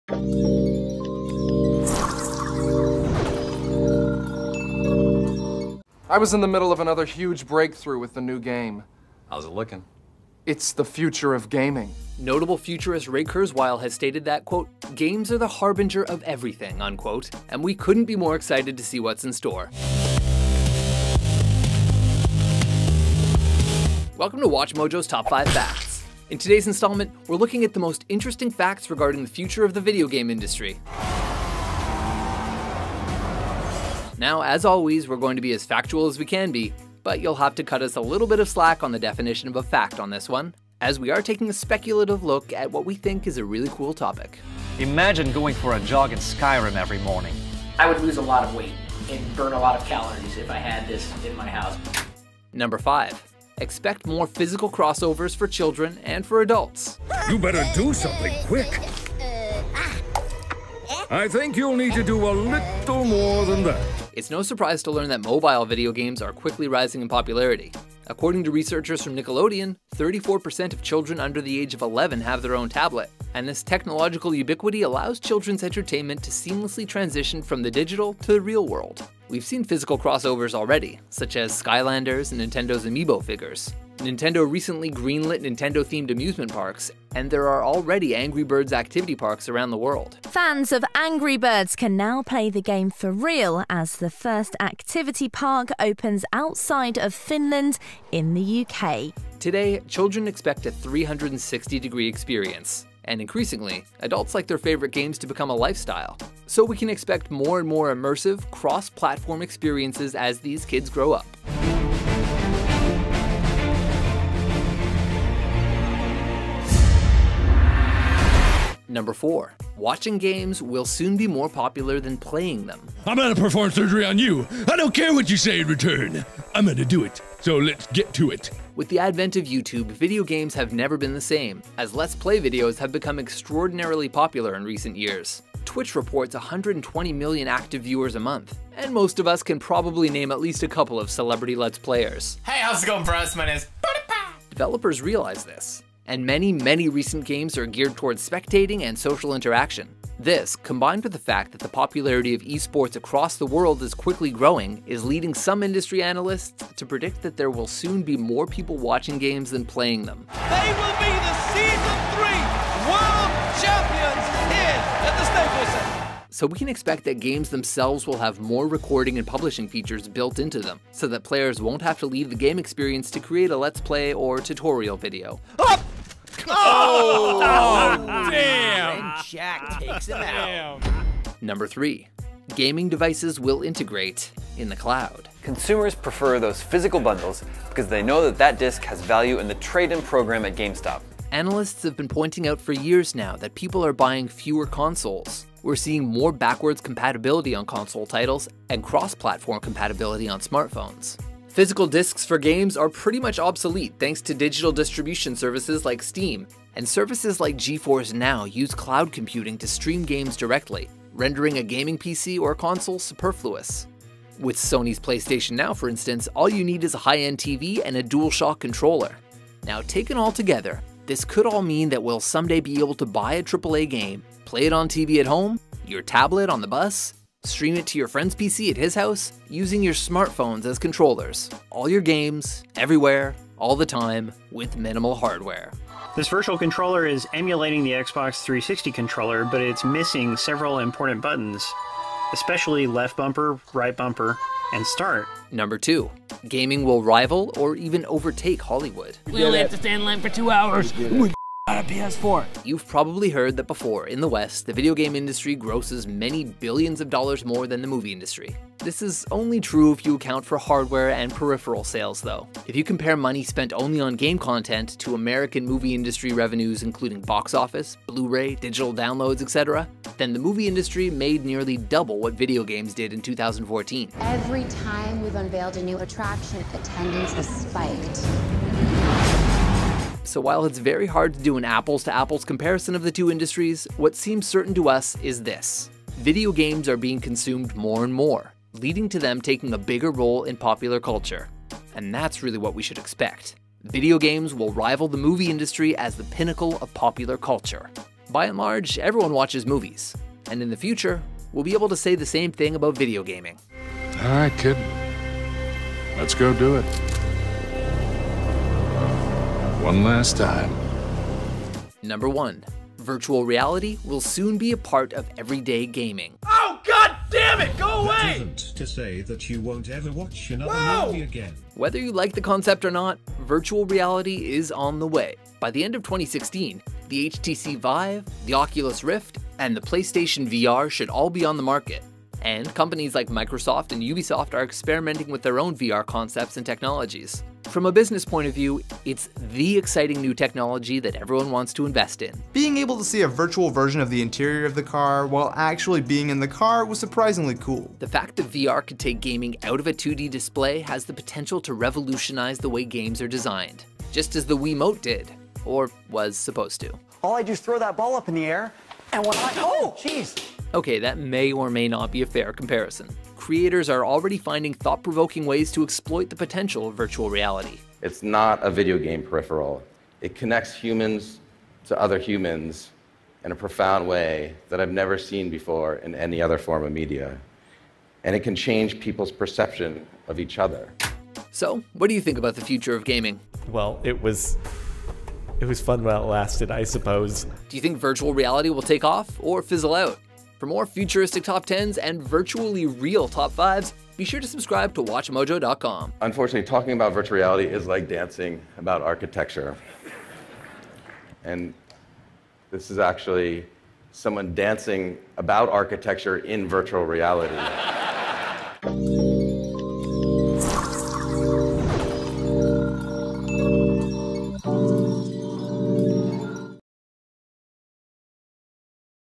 Top 5 Facts about the Future of Video Gaming
Notable futurist Ray Kurzweil has stated that, "Games are the harbinger of everything," and we couldn't be more excited to see what's in store. Welcome to Watchmojo’s top five facts!

In today's installment, we're looking at the most interesting facts regarding the future of the video game industry.

Special thanks to our user nathansharp28 for submitting the idea on our Interactive Suggestion Tool